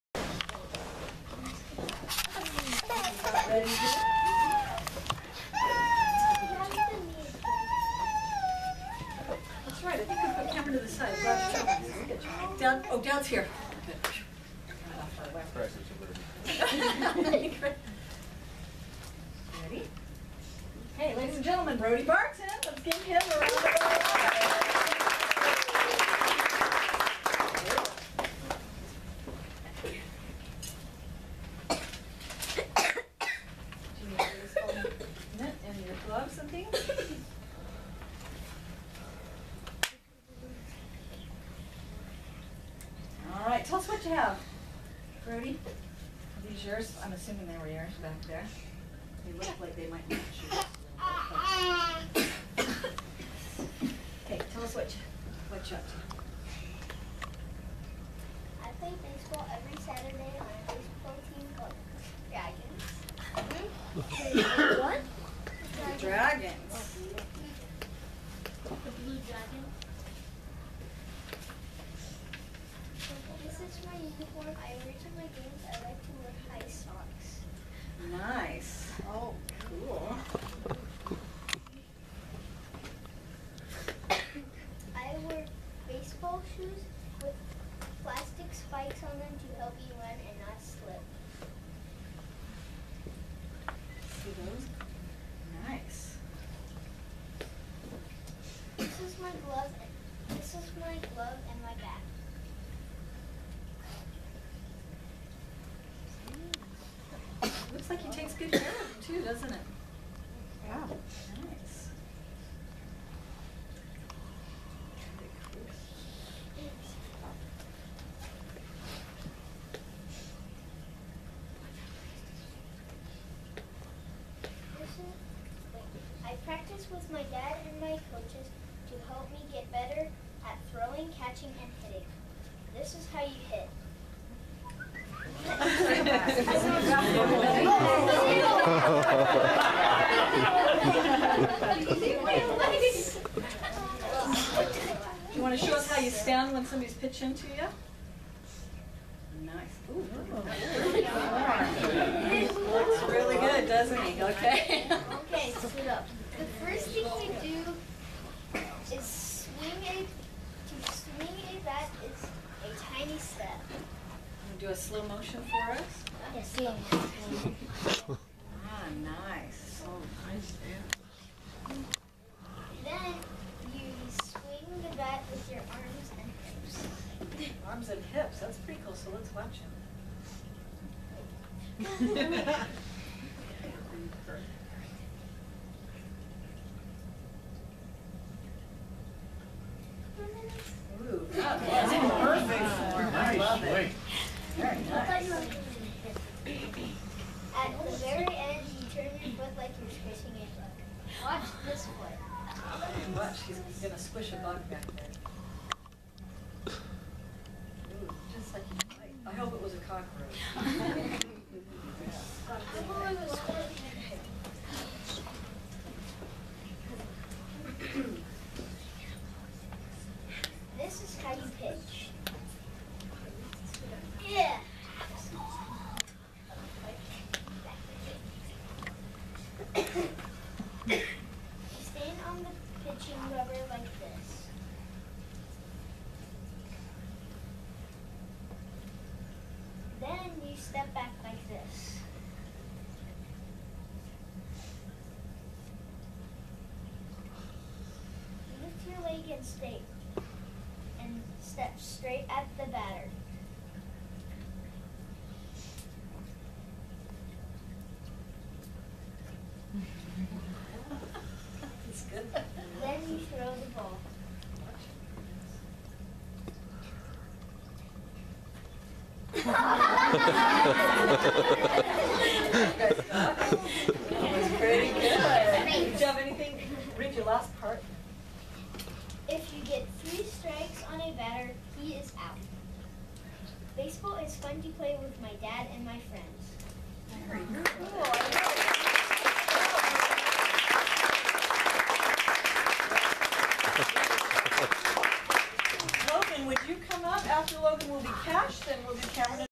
Brody Talent Show
Brody showing off his baseball skills at his 2nd grade talent show